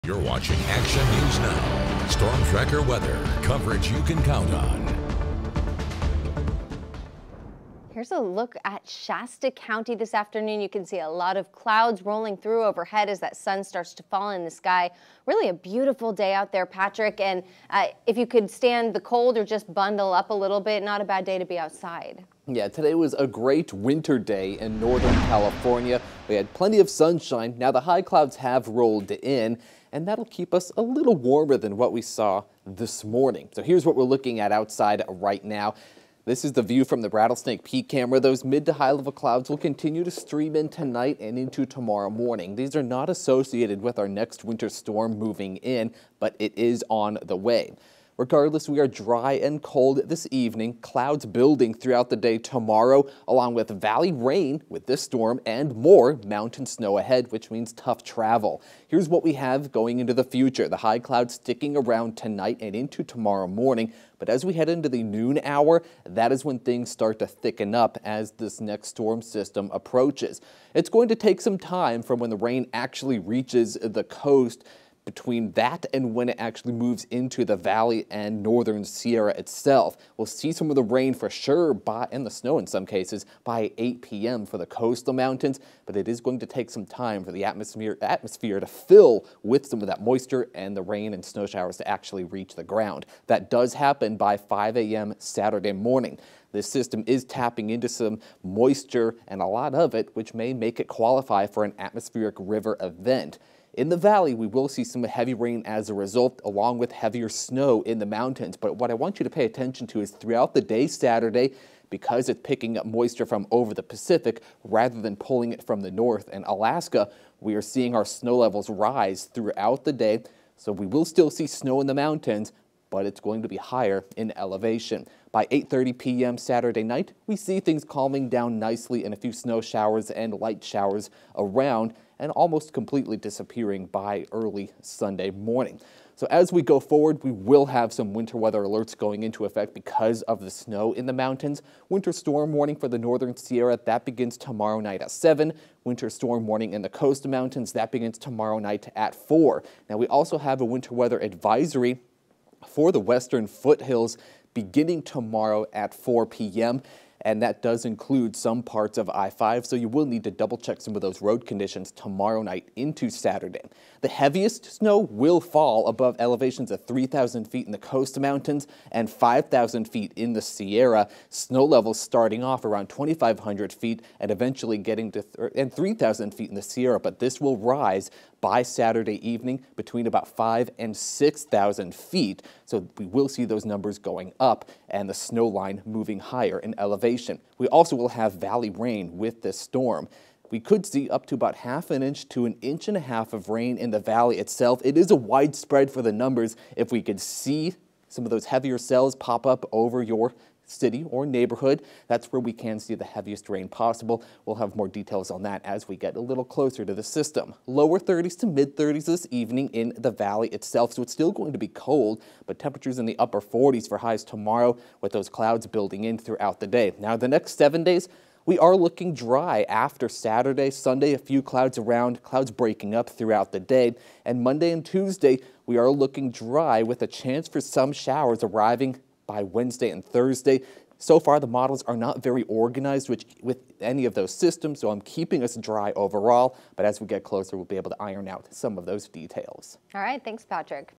Another Winter Storm Around The Corner
Another winter storm is just around the corner. Cold weather greets us tomorrow before clouds thicken up with rain for the valley and heavy mountain snow arriving late tomorrow night into Saturday.